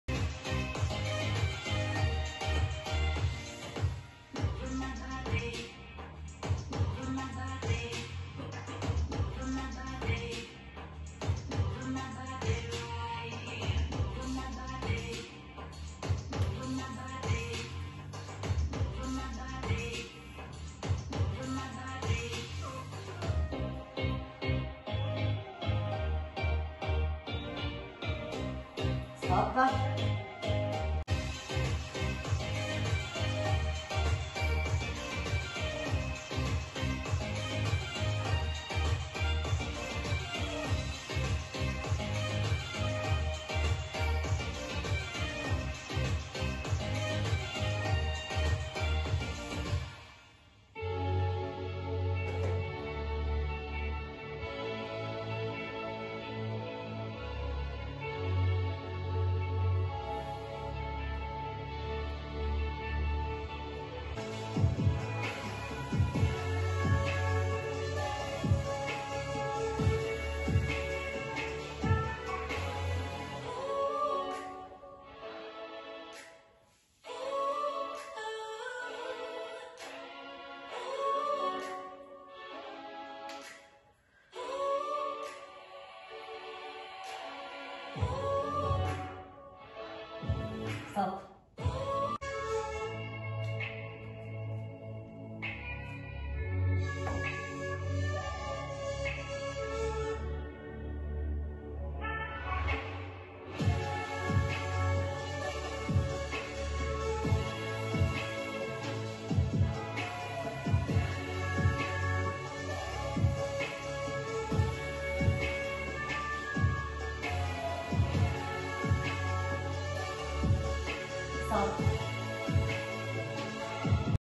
MUOVITI con PIGRILATES Pilates per Pigri: 4 Mini Esercizi Total Body in quadrupedia!
Hai poco tempo? Troppo pigra? Ti bastano 2 minuti di tempo per tonificare il tuo corpo in posizione di QUADRUPEDIA, con gli esercizi di Pilates!!!
Per altri video di MUOVITI CON PIGRILATES, visita il mio sito internet